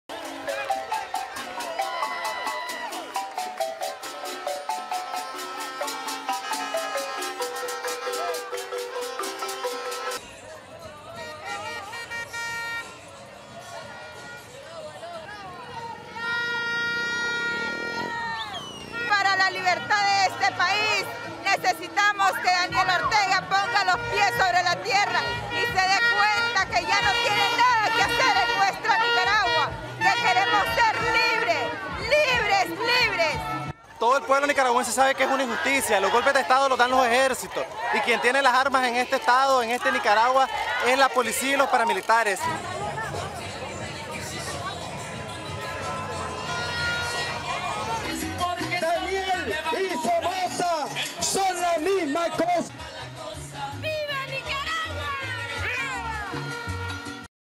Thousands Walk In Nicaragua Demanding Ortega Step Down
August 16, 2018 - Thousands of Nicaraguans march through the streets of the capital Managua demanding that President Daniel Ortega, who has been in power for the last 11 years, step down and that anti-government protesters jailed over the past four months be released.